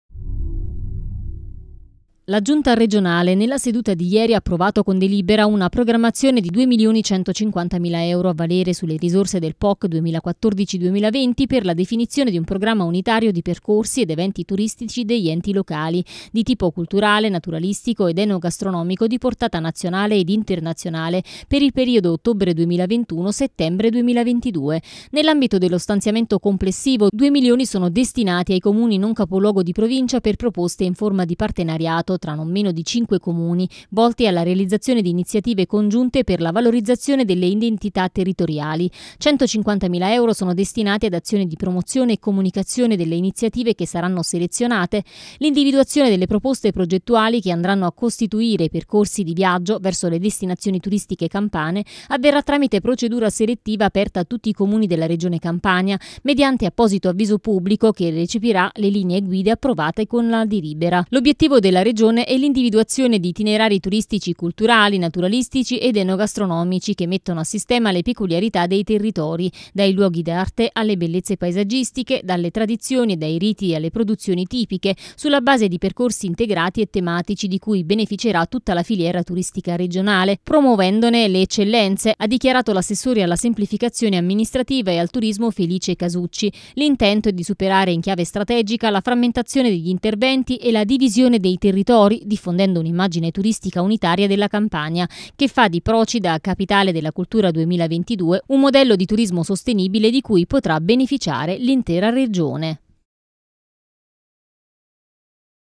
Campania, nuovi percorsi turistici per gli enti locali: Approvato il programma per oltre 2 milioni
La giunta regionale, nella seduta odierna, ha approvato con delibera una programmazione di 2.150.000 euro a valere sulle risorse del POC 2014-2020 per la definizione di un Programma unitario di percorsi ed eventi turistici degli enti locali, di tipo culturale, naturalistico ed enogastronomico di portata nazionale e internazionale, per il periodo ottobre 2021- settembre 2022.